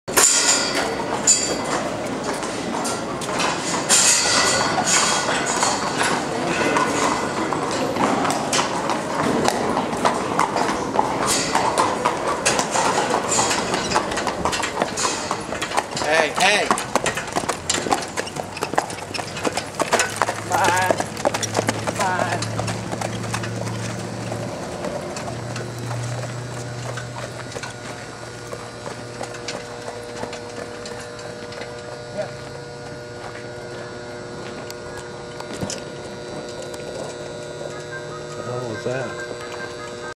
Island Heat - Harness Race Horse: Post Parade at Balmoral Park Race Track
2/14/10 Balmoral Park in Chicago - Island Heat was tough to handle tonight going onto the track. She finished 2nd.